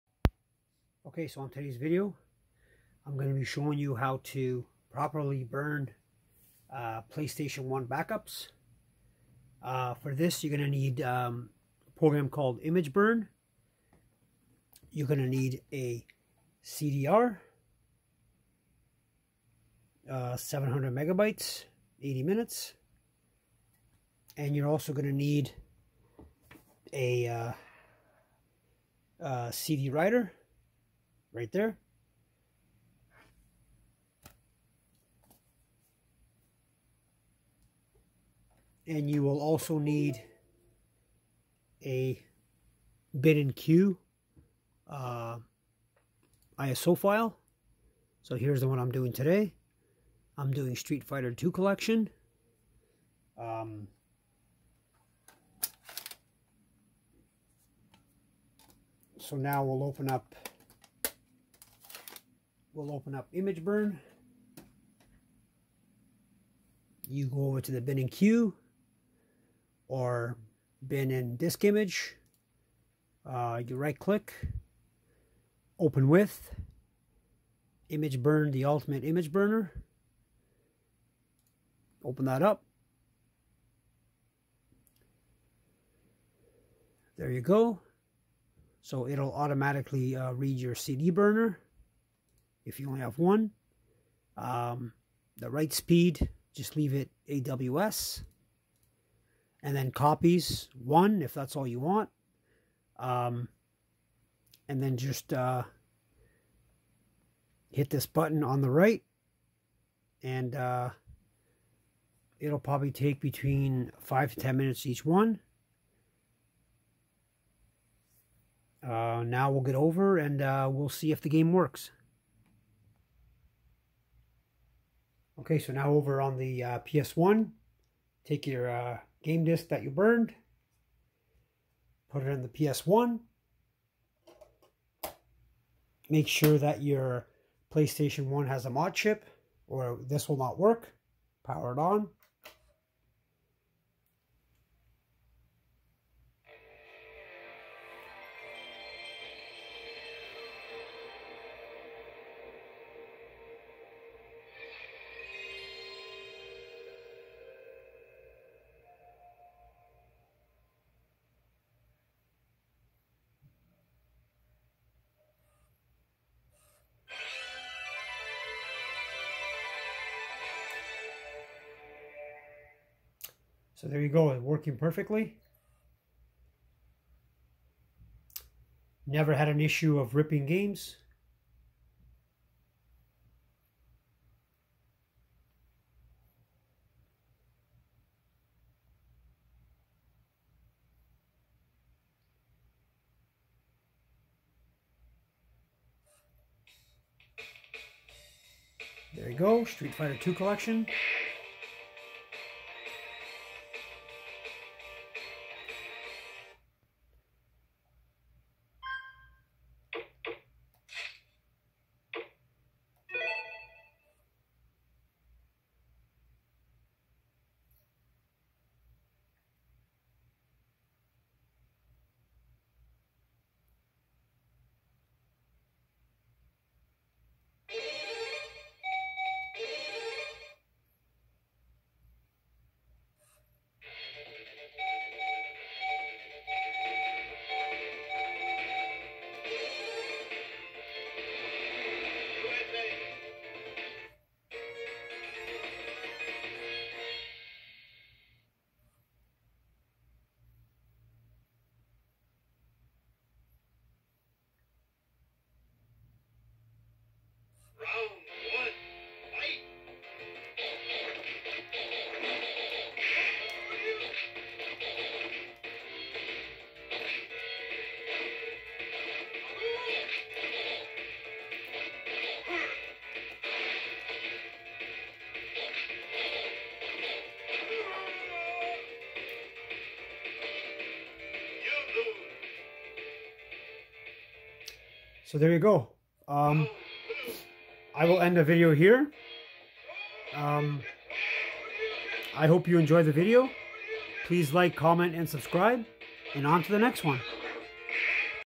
How to Burn Playstation 1 Backups! ISO to CD-R.
Burn Playstation 1 Backups! ISO to CD-R using imgBurn!
* Make sure your using a modded Ps1 with chip & CD-R Dics 700MB / 80 Min.
* Games should be in Bin and cue format!